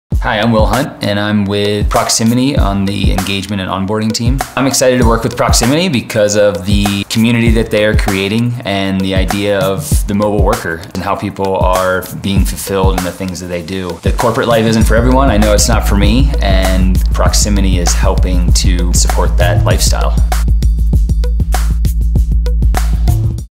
Proximity Engagement and Onboarding Coordinator Will Explains His WHY
Proximity Engagement and Onboarding Coordinator Will Hunt explains his WHY for being a part of the Proximity team.